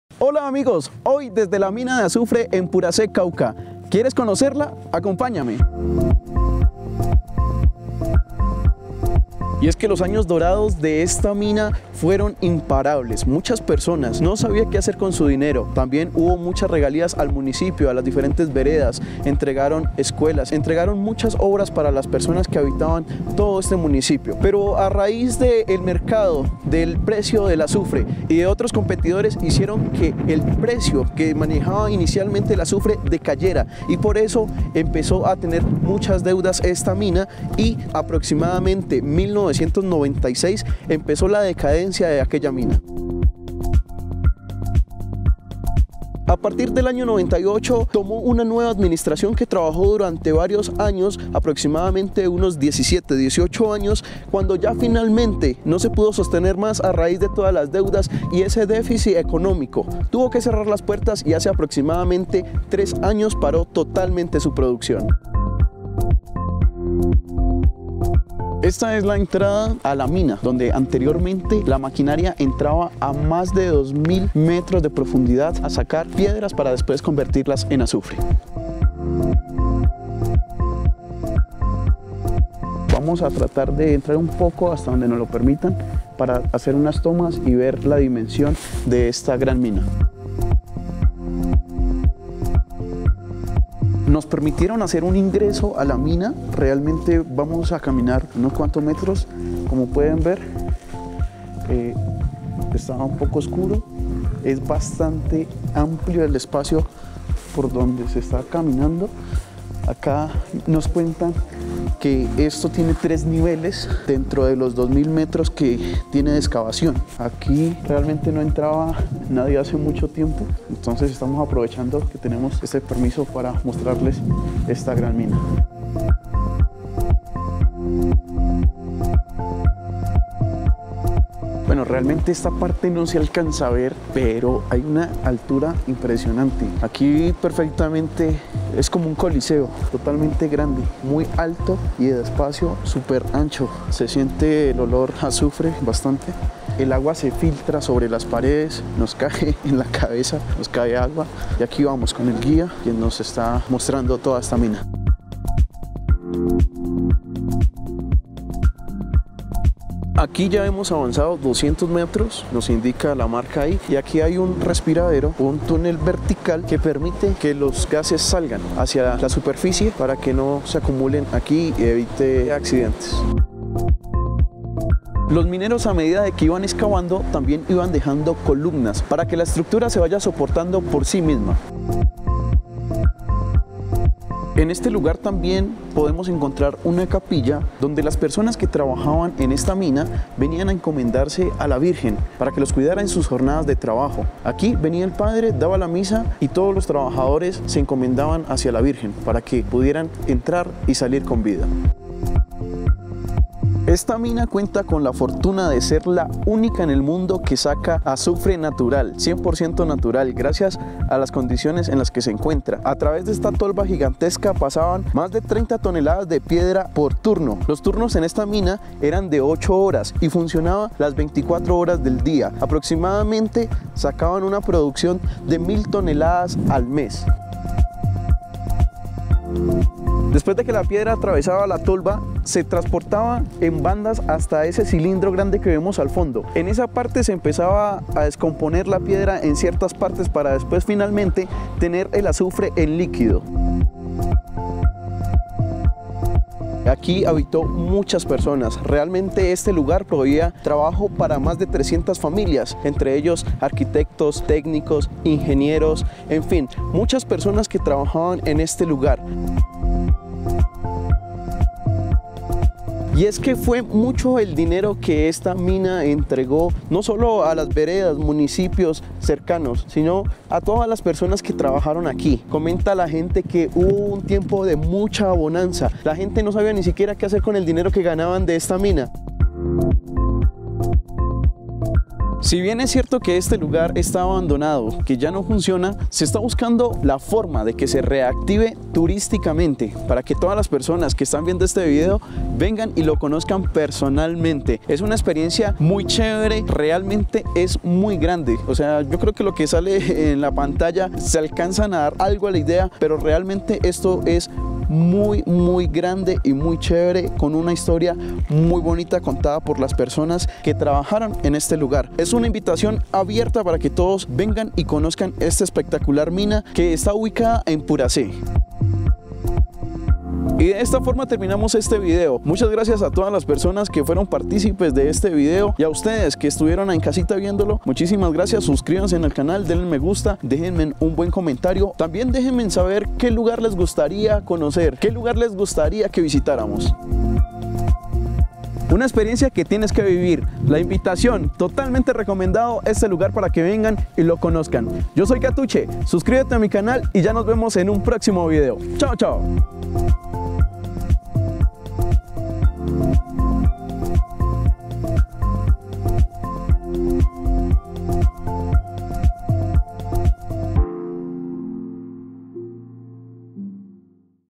Mina de Azufre - Puracé Cauca // CatucheTV
Conoce la Mina de Azufre de origen volcanico, ubicada a 3.600 metros de altura sobre el nivel del mar, en el Municipio de Puracé Departamento del Cauca, a 50 kilómetros de la ciudad de Popayán.